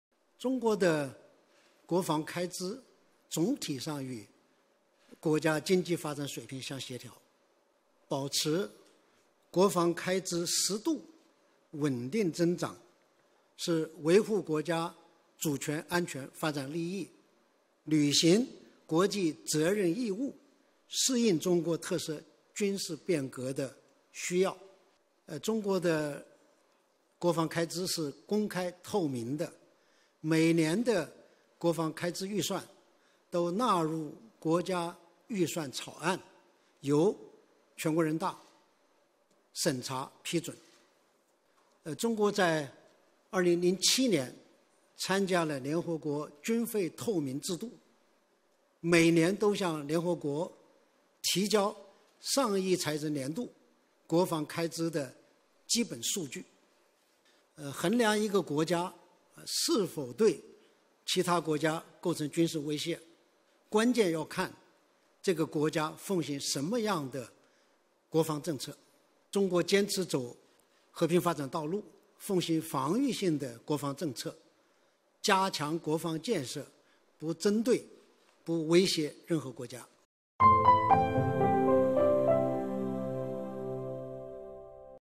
张业遂：中国加强国防建设不针对不威胁任何国家
十三届全国人大四次会议于3月4日在人民大会堂新闻发布厅举行新闻发布会，由大会发言人张业遂就大会议程和人大工作相关问题回答中外记者提问。
        在回答记者提问“今年中国国防支出会不会大幅增长？增幅将是多少？中国军力发展是否对其他国家构成威胁？”问题时。
        张业遂表示，中国的国防开支总体上与国家经济发展水平相协调。保持国防开支适度稳定增长，是维护国家主权、安全、发展利益，履行国际责任义务，适应中国特色军事变革的需要。中国的国防开支是公开透明的。每年的国防开支预算都纳入国家预算草案，由全国人大审查批准。中国在2007年参加了联合国军费透明制度，每年都向联合国提交上一财政年度国防开支的基本数据。衡量一个国家是否对其他国家构成军事威胁，关键要看这个国家奉行什么样的国防政策。中国坚持走和平发展道路，奉行防御性国防政策，加强国防建设不针对、不威胁任何国家。（制作 王嘉怡）